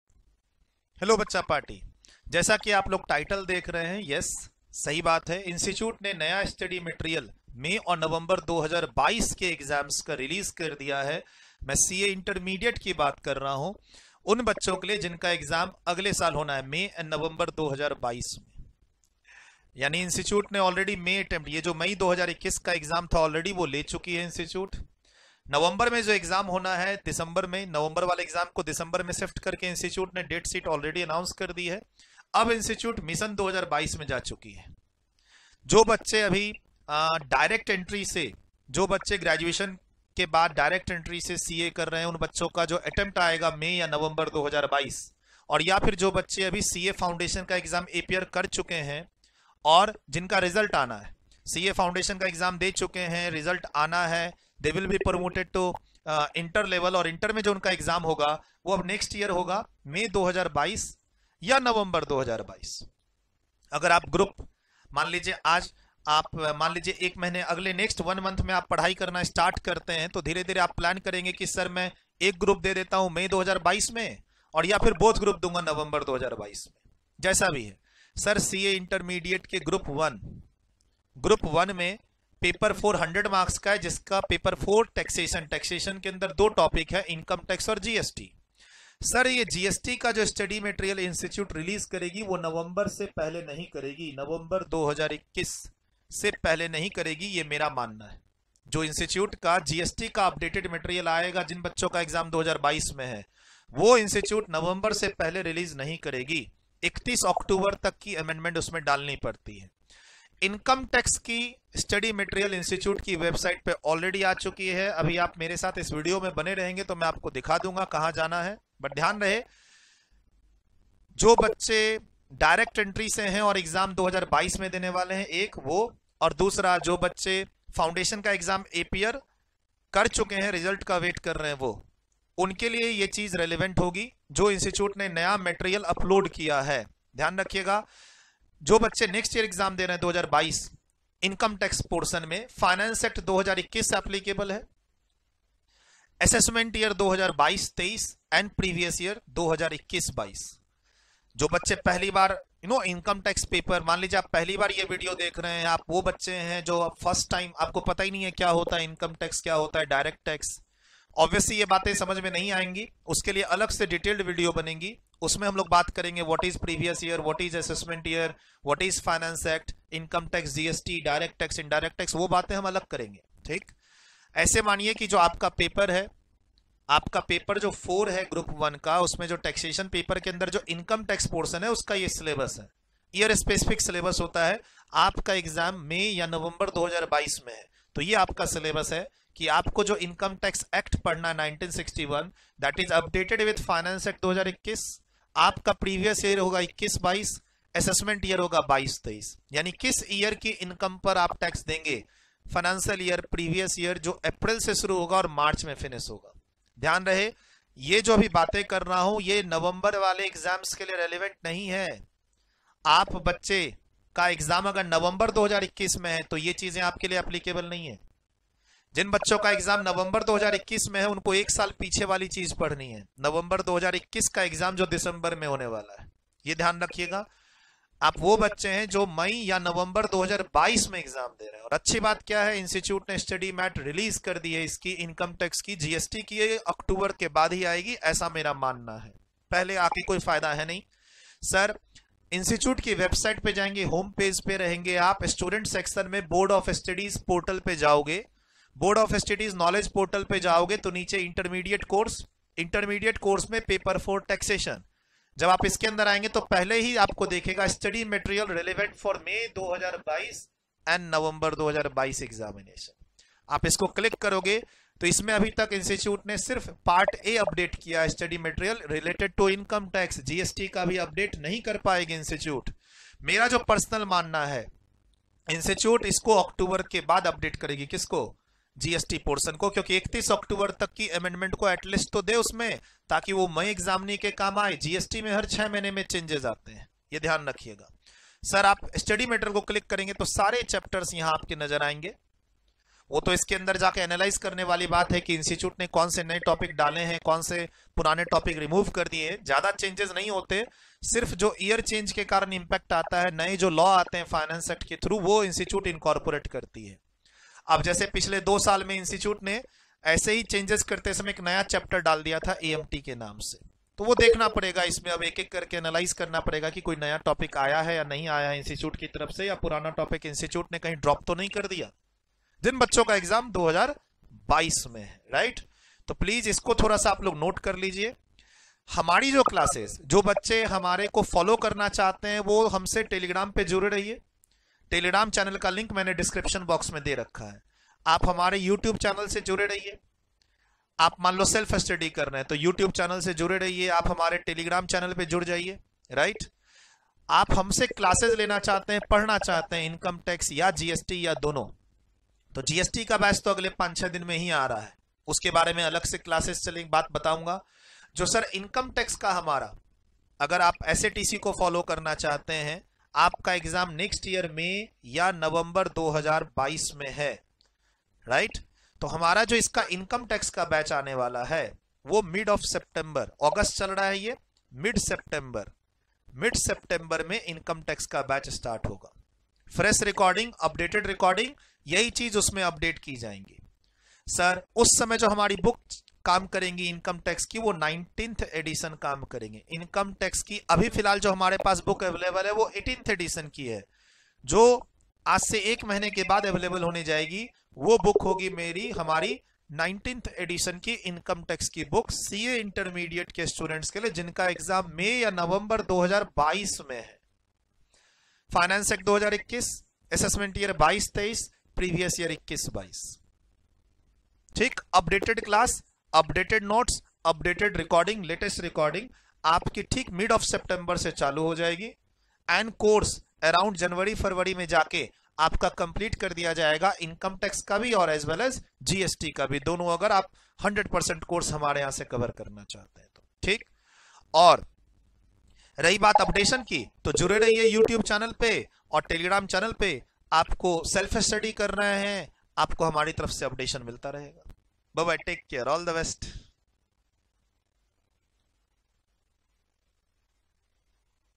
BREAKING NEWs | ICAI | CA-INTERMEDIATE | NEW INCOME TAX STUDY MATERIAL | ASSESSMENT YEAR 2022-23
FOR CA STUDENTS APPEARING IN MAY/NOV 2022 EXAM

May/Nov 2022 Exam
Assessment Year 2022-23
Finance Act 2021